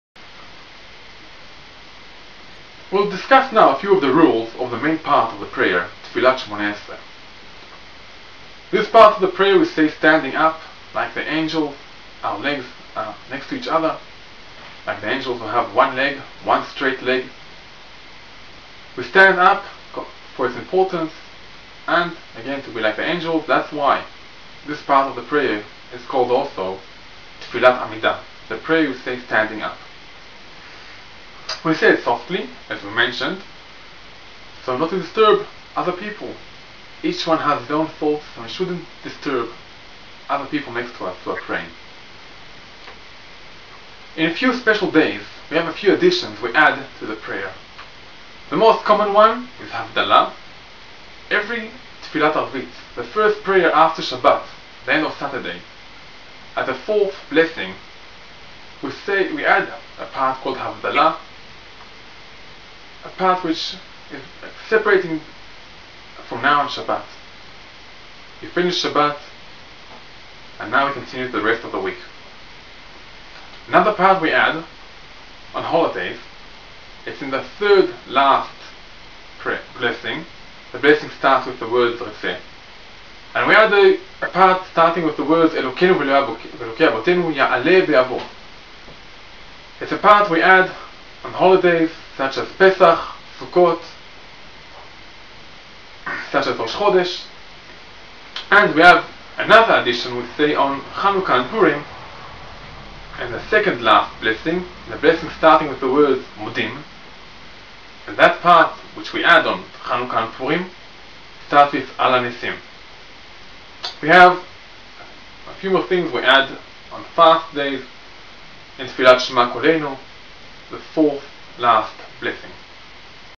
How to feel home in the Synagoue 8
About the Amidah - the main part of the prayer
Some Laws of the Amidah

2 minutes of Judaism with R. Yisrael Wende